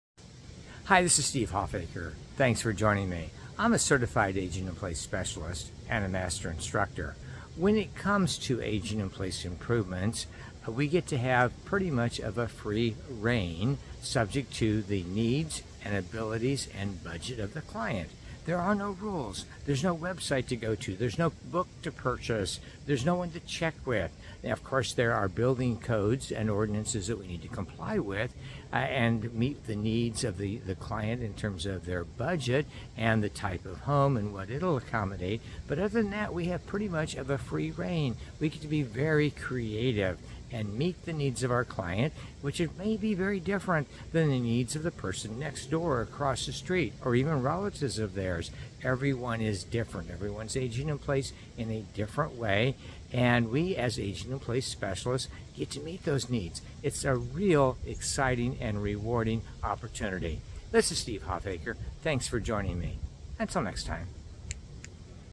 Steve Hoffacker, CAPS - "No Rules, Just Results"
Award-winning Certified Aging In Place Specialist Master Instructor Steve Hoffacker, CAPS, celebrates that aging in place renovations do not need to follow any specifications or rules other than local applicable building codes and that we get to design and complete renovations for our clients to meet their needs and requirements rather than a regulatory source. The client's budget, needs, and desires primarily dictate what we do to help them.